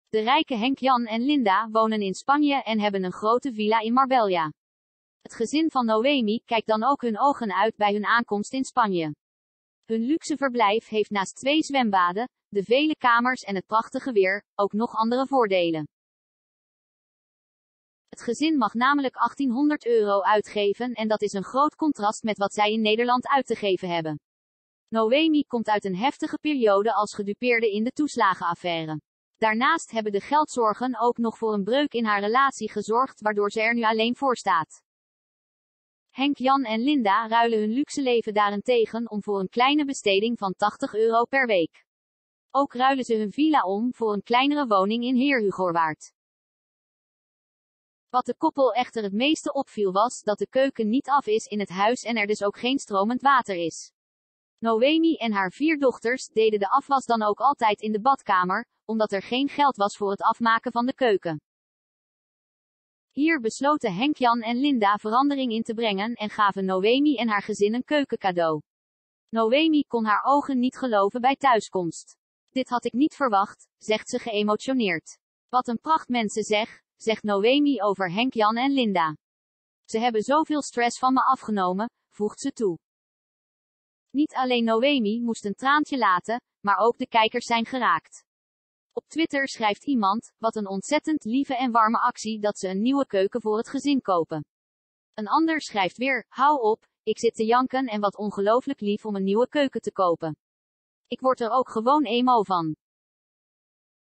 Gedupeerde moeder Steenrijk, Straatarm ontroerd door gift van rijke gezin
Gisteravond was er weer een emotionele aflevering te zien van het SBS-programma 'Steenrijk, Straatarm'. Hierin maakten de kijkers kennis met Noëmi en haar vier dochters: Hailey, Isabella, Noëlia en Romy. Breed hebben ze het niet, want het gezin uit Heerugowaard moet rondkomen met zo'n 80 euro per week. Dit komt onder andere doordat Noëmi gedupeerde is van de toeslagenaffaire. De rijke Henk Jan en Linda hebben bij vertrek een mooie cadeau achtergelaten voor dit arme gezin.